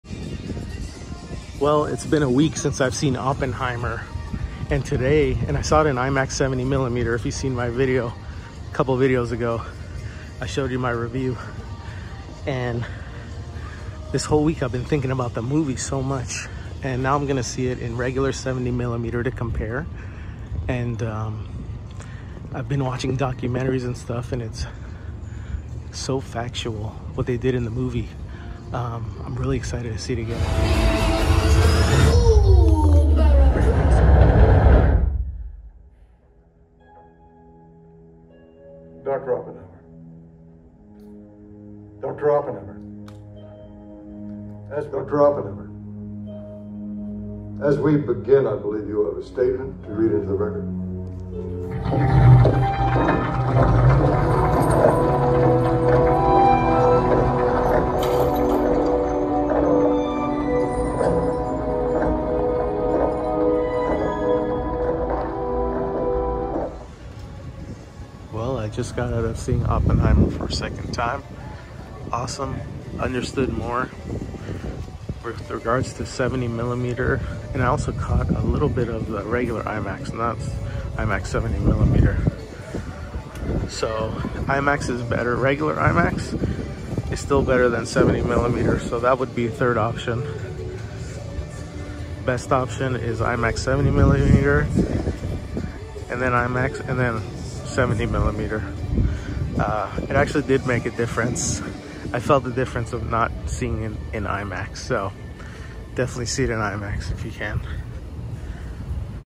Comparing Formats | Oppenheimer IMAX 70 mm VS 70 mm VS Standard IMAX ￼
I finally saw Openheimer in all three formats
70 mm
standard IMAX (dual laser 1.90 “liemax”)
 IMAX 70 mm (1.43)

Here is the answer to ￼which version is best to worst.